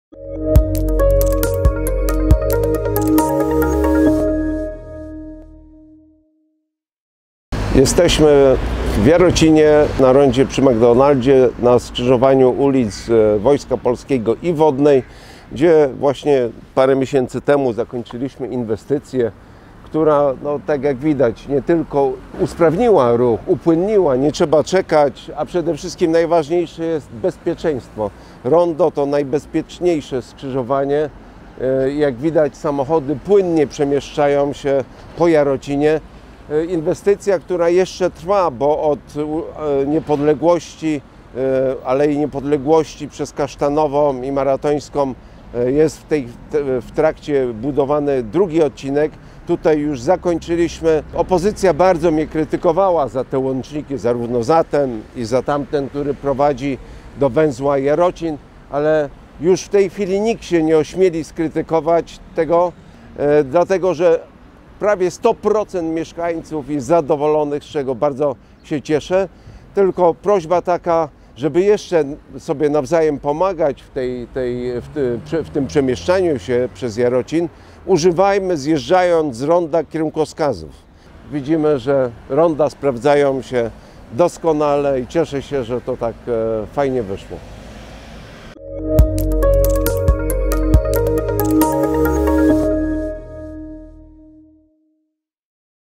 Ronda sprawdzają się doskonale
Rondo, które powstało w ubiegłym roku na skrzyżowaniu ul. Wodnej i Wojska Polskiego jest jednym ze skrzyżowań o ruchu okrężnym, które realizowane są w ramach przebudowy ciągu ulic Kasztanowa, Do Zdroju, Wrzosowa, Maratońska, Św. Ducha oraz Poznańska i Wojska Polskiego w Jarocinie.   - Rondo, to najbezpieczniejsze skrzyżowanie – podkreśla burmistrz Jarocina, Adam Pawlicki. – Inwestycja  zdecydowanie upłynniła w centrum miasta – dodaje włodarz gminy.
Ogromne przedsięwzięcie realizowane przez samorząd Gminy Jarocin będzie kosztowało 25 mln zł. Inwestycja realizowana jest przy wsparciu dotacji w wysokości 16 mln zł, pozyskanej z Funduszu Dróg Samorządowych.
Przypomnijmy, że w ramach I etapu inwestycji, w ubiegłym roku wybudowane zostały 2 ronda. Poza wspomnianym przy ulicach: Wodnej i Wojska Polskiego, powstało także rondo na skrzyżowaniu ul. Wojska Polskiego z ul. Św. Ducha. Przebudowana też została droga pomiędzy rondami i jej fragment w kierunku Al. Niepodległości.
W tym roku realizowany jest drugi etap tej ogromnej inwestycji. Wykonuje go firma Polimex Mostostal S.A. Tegoroczne prace rozpoczęły się na odcinku ul. Maratońskiej pomiędzy ul. Św. Ducha i Żerkowską. Stopniowo prace przesuwać się będą ul. Maratońską w kierunku ul. Kasztanowej. Kolejne drogi zyskają nową nawierzchnię jezdni oraz chodnika, a także ścieżkę rowerową. Wykonane zostanie także oświetlenie uliczne typu LED na ulicach: Kasztanowa, Do Zdroju i Maratońska (od ul. Wrzosowej do ul. Św. Ducha) oraz remont istniejącego kanału deszczowego w części ulic Do Zdroju, Maratońska i Wrzosowa. Finałem będzie budowa kolejnego ronda - na skrzyżowaniu ul. Poznańskiej i Kasztanowej.